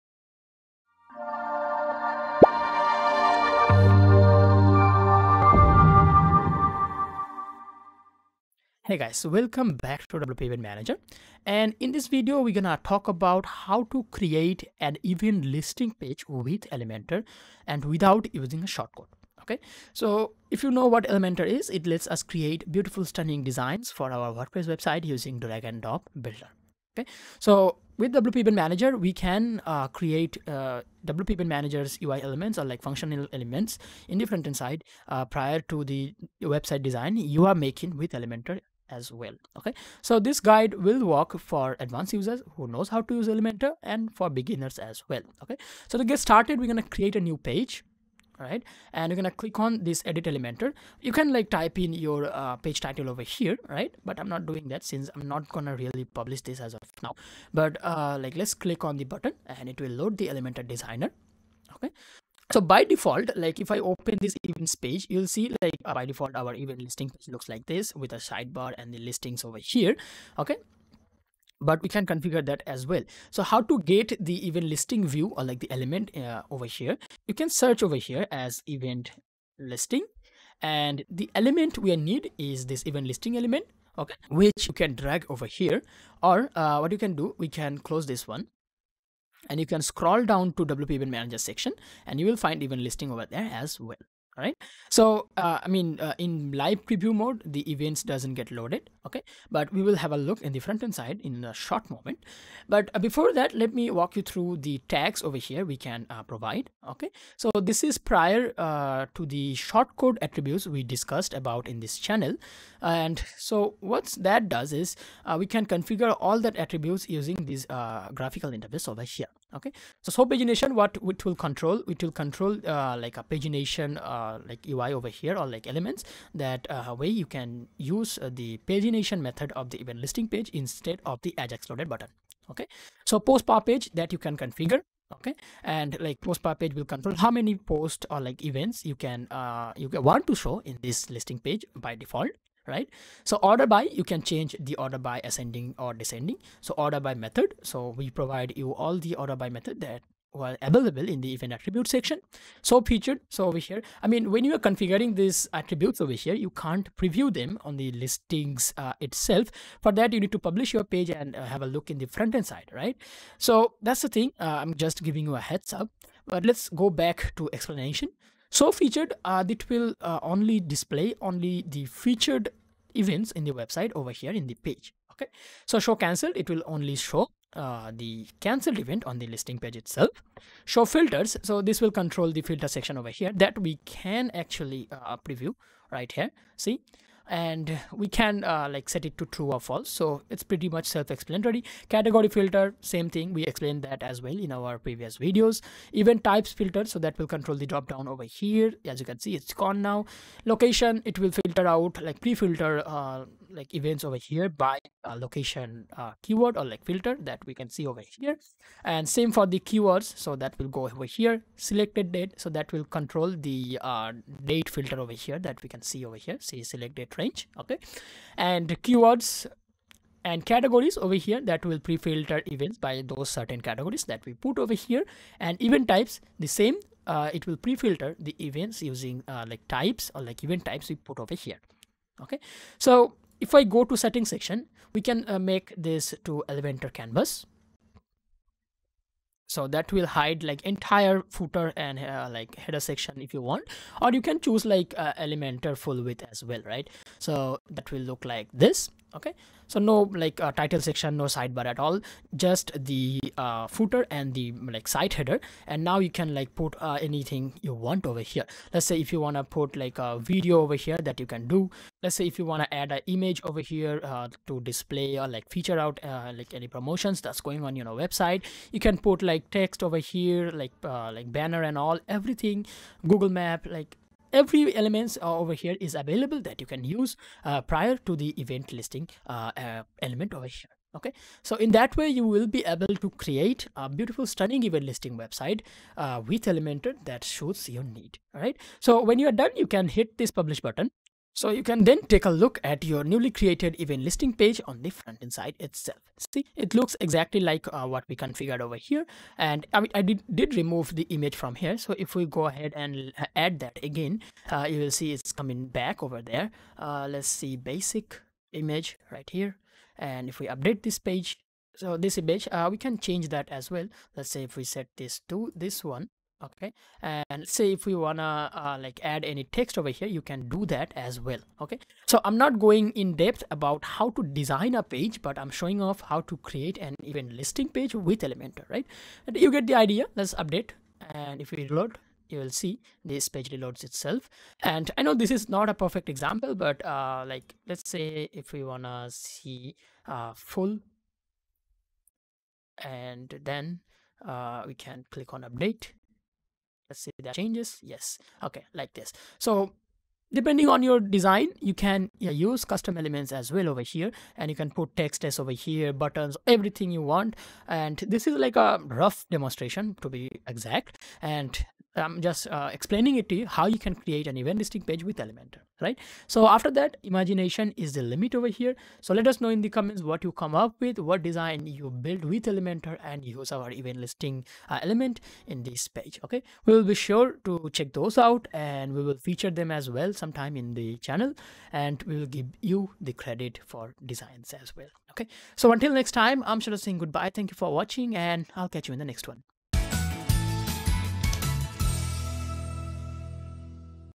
How To Create Event Listing Page With Elementor In WordPress
Are you very new to WordPress and looking out for ways to set up your page? Try Elementor!

It is one of those free plugins that help you customize your website page very easily.

Elementor works seamlessly with almost any WordPress theme.

The following steps will enable an individual to install the Elementor plugin:
1. Go to Dashboard -click on- Plugins -click on- New Plugin.
2. Next search for the Elementor plugin in the search box.
3. Post which you need to install and activate the “Elementor” plugin.

Since we have now installed this plugin, we now need to build the event listing page. Working on the template with the Elementor plugin is easy. Hence, the new page so created needs to now be edited with the Elemnetor plugin.

Please follow the below-mentioned steps to edit your page.

4.A new page where all the event listings need to be displayed needs to be created. To do that, go to Admin -click on- Pages -click on- Add new.
5. Enter the name of the page “Event List” and edit it with Elementor.
6. You will be redirected to drag and drop desks using which the page now needs to be built. Under the search option, search for Event List and drag the widget to the page.
7. Setup widget setting as per your requirement and update.
8. After the update, you can now see the changes on your actual page.